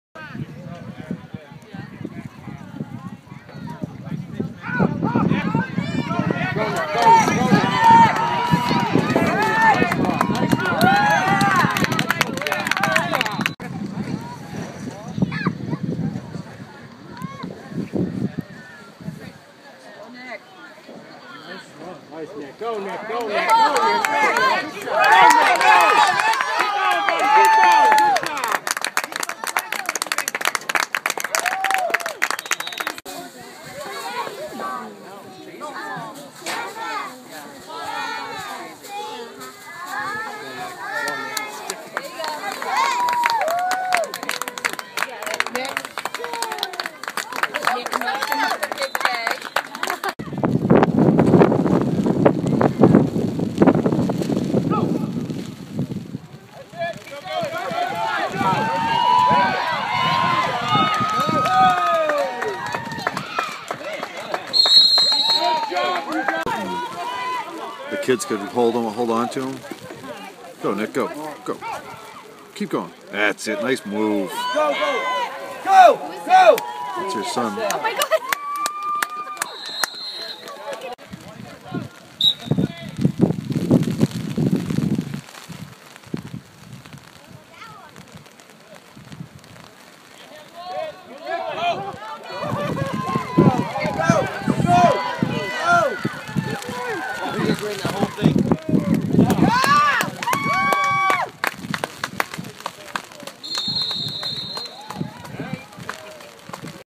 nickhighlights2
Bunch of tds for Nick over the last two games of the year. Good moves Nick.